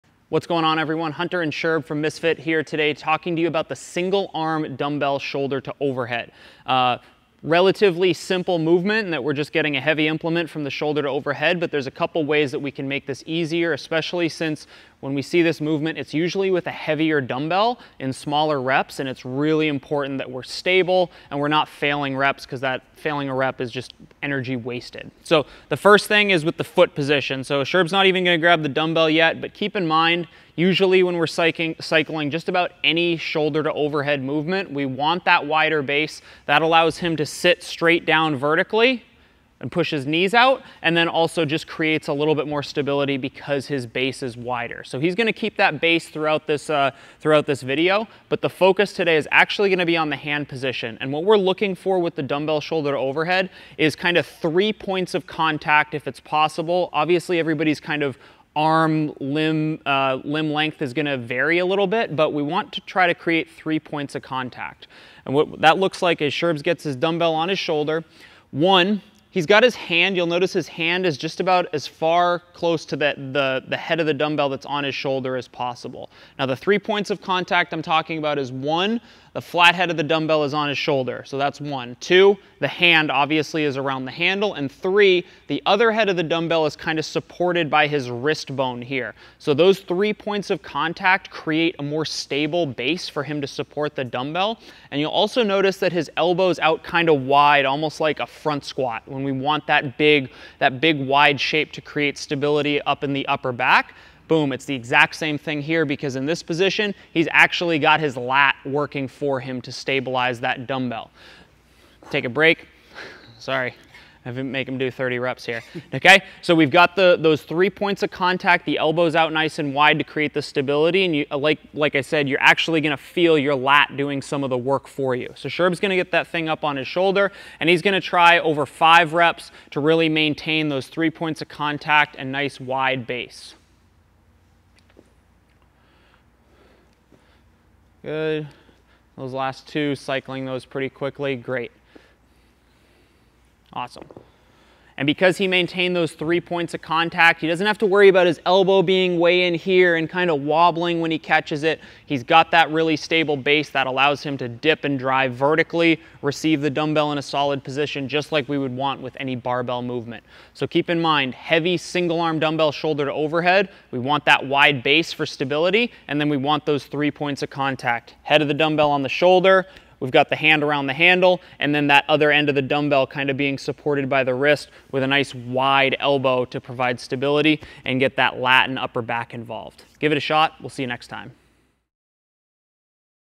Single Arm Dumbbell Shoulder to Overhead Tips!
We don't often program the Single DB Shoulder to OH, but when we do, you know it's a spicy one. Hunter and Sherb give you some tips on how to successfully handle a single, heavy, unwieldy dumbbell in today's short chipper.

Get a Discount on Pure Spectrum CBD!!